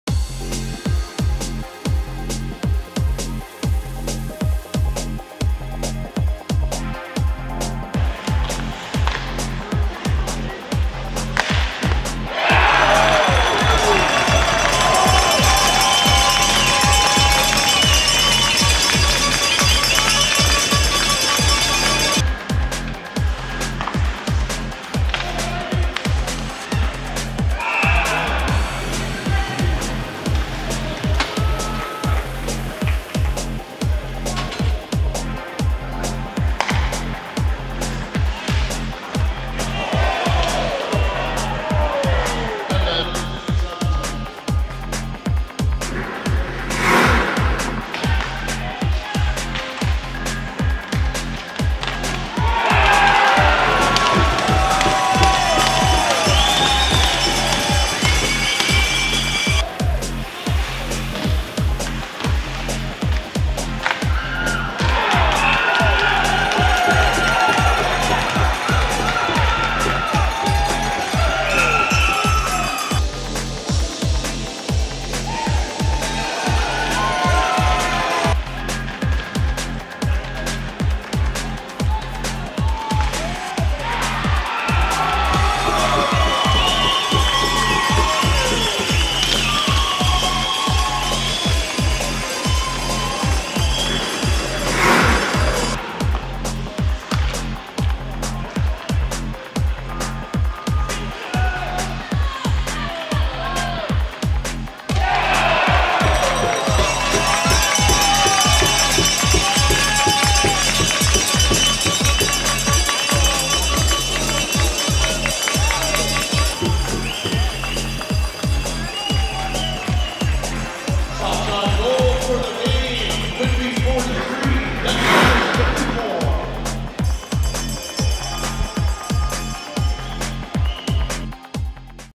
Oct 24 2009 vs Dundas
Anthony Marshall stopped all but one of the 34 Dundas shots he faced as the Dunlops cruise to a 5-1 victory over the 2009 Champions Dundas Real McCoys.